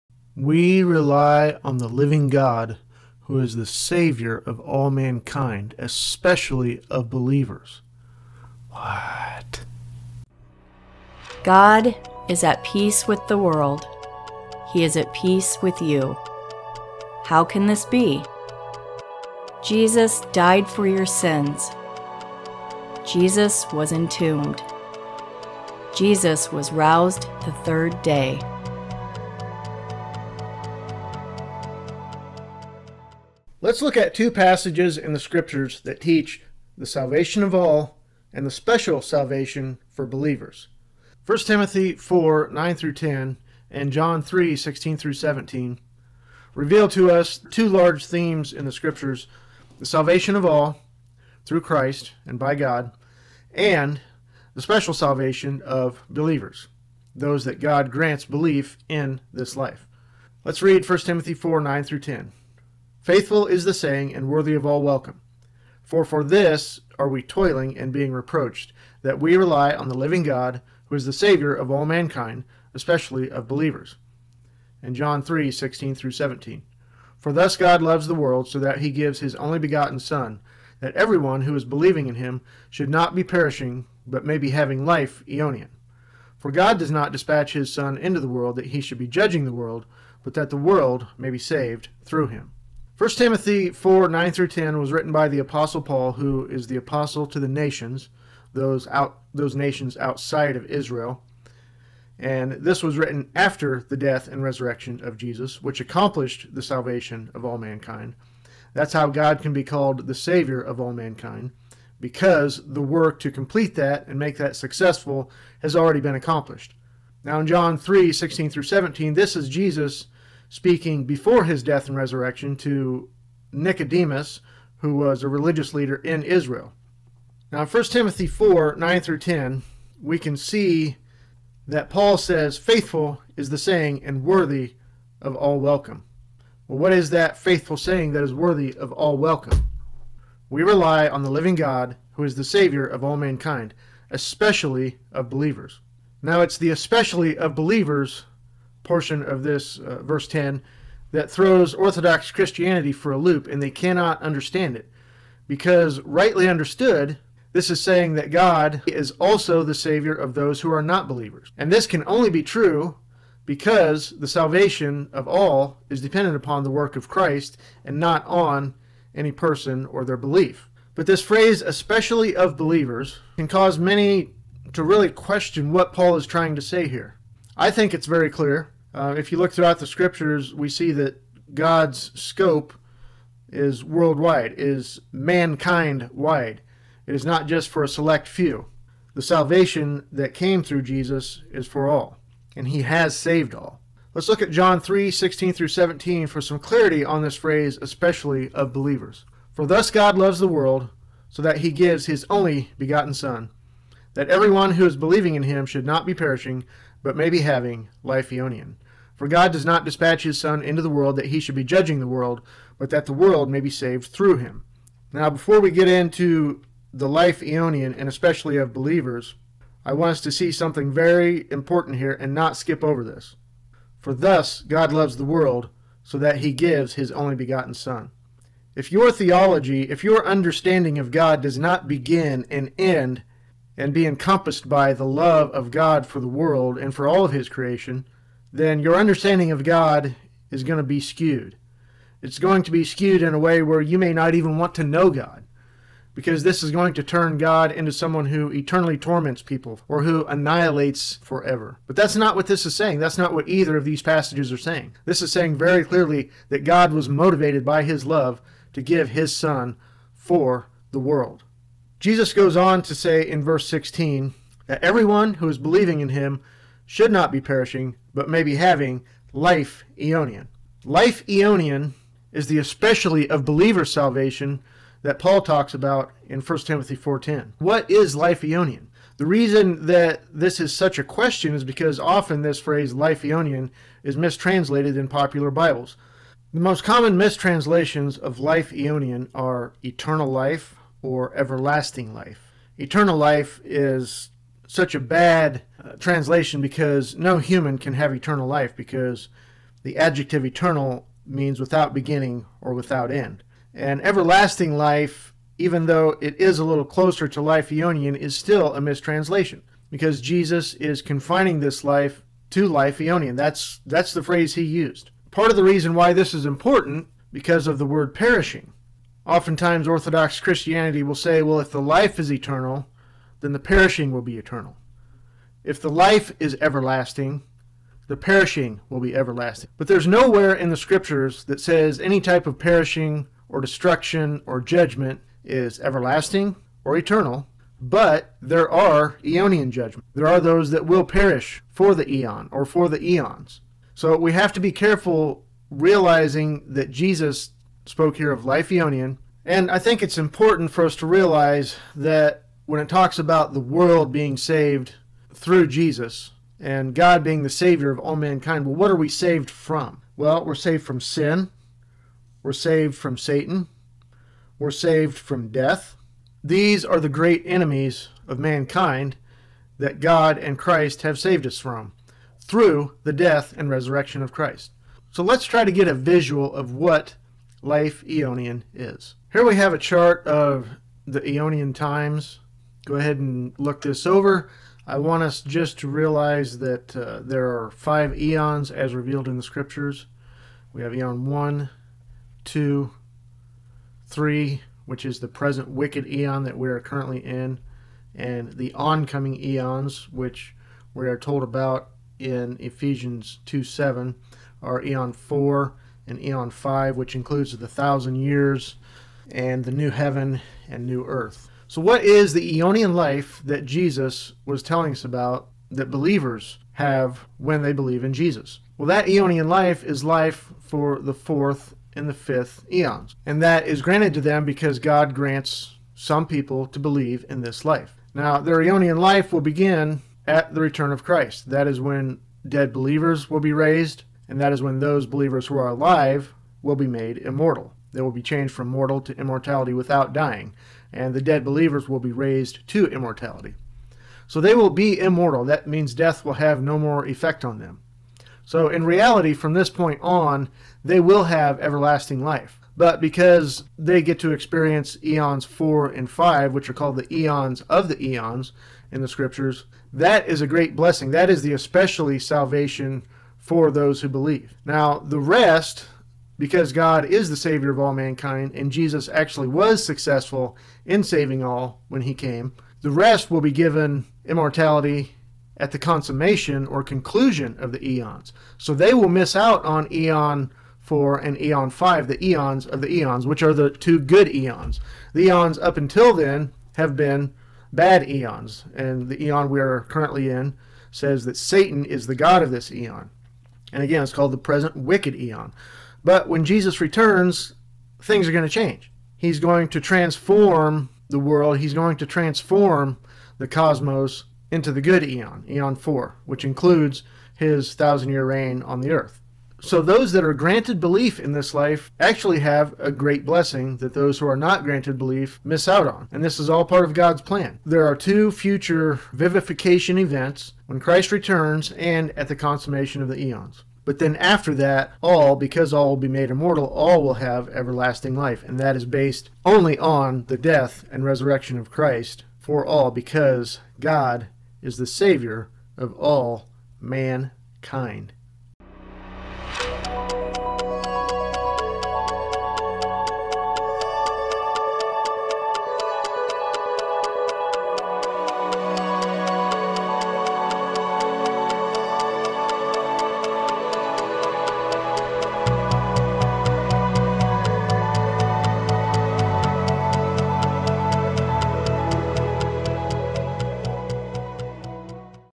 "How John 3:16 sheds light on 1 Timothy 4:10."
God, the Savior of all mankind, has something special in store for those He grants belief to in this life. It's an early experience of immortality. It's an enjoyment of the two good oncoming eons. This does not mean the rest of humanity will burn in hell forever (that's a Christian myth). They will simply be immortalized later. All people--those who start the party, and those who join later--will have everlasting life, based solely on the work of God and Jesus.

Musician: ASHUTOSH